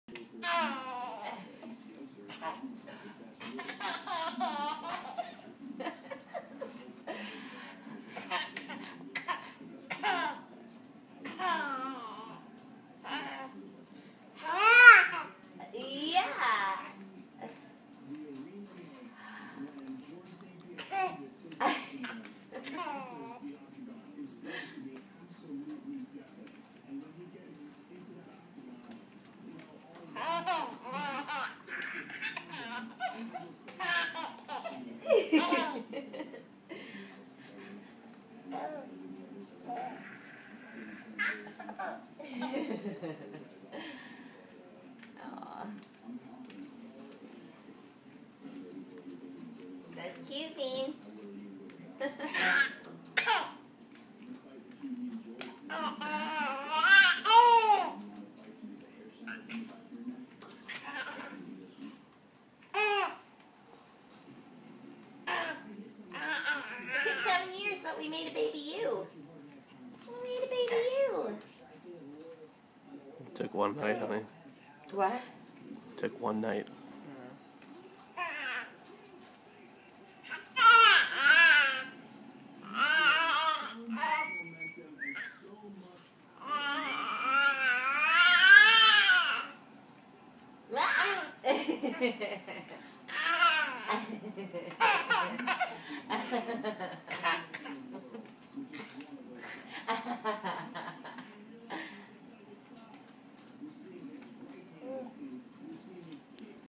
Jolly Jumper Tickles
No description necessary. Hearing your own child's laughter is heaven.